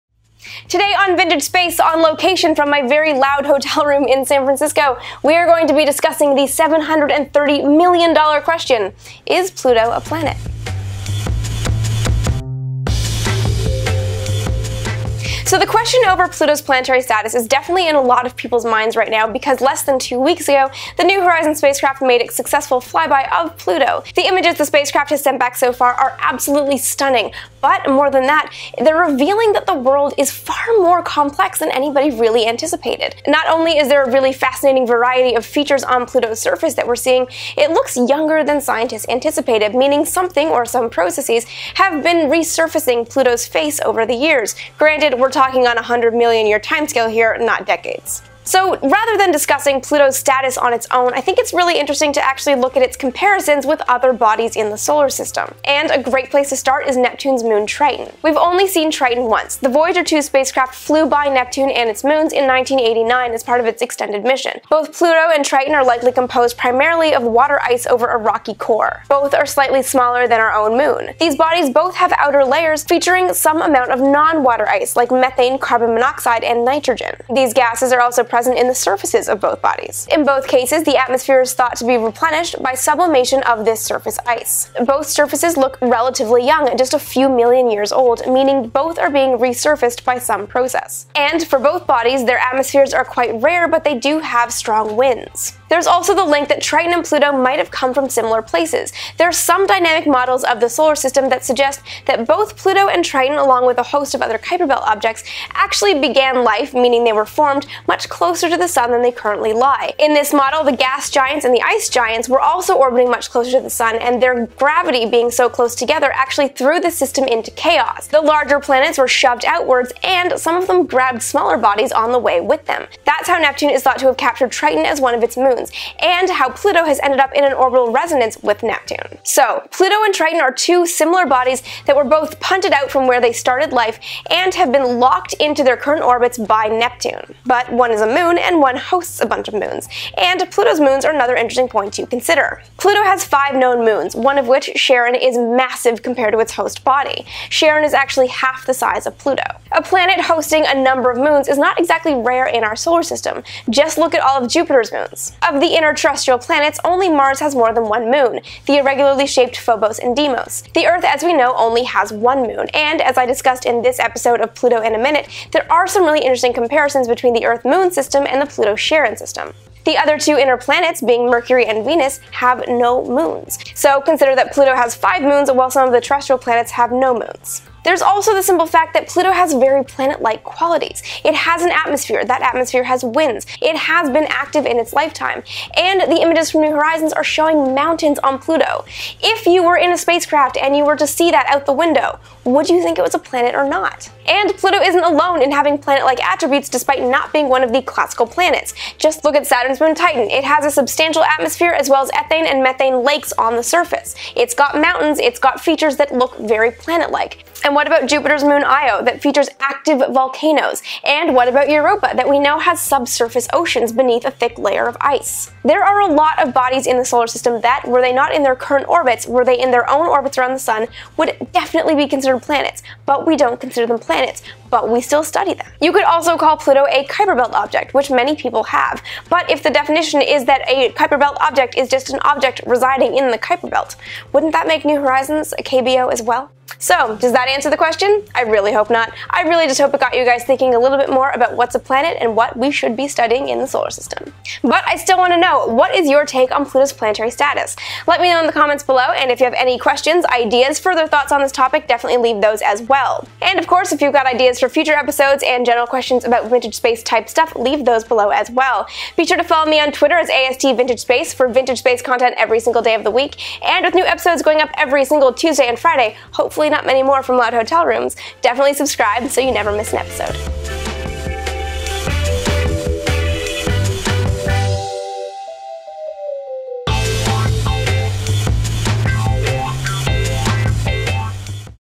Is Pluto a Planet Yet?
It's the question on everyone's mind two weeks after New Horizons flew by Pluto: is it a planet or isn't it?

For the layperson, the question of Pluto's planetary status is an emotional one. People of a certain generation (namely mine) grew up with nine planets and were devastated when the planet was demoted to dwarf status in 2006. For others in the planetary sciences community, the change in moniker brings implications about validity of their work and potential funding sources -- is a planetary scientist studying Pluto suddenly not a planetary scientists after the body's demotion?

The conversation about Pluto's planetary status is interesting and can go a number of ways. Because Pluto can't totally be taken out of context from the bodies with which it shares a cosmic neighbourhood.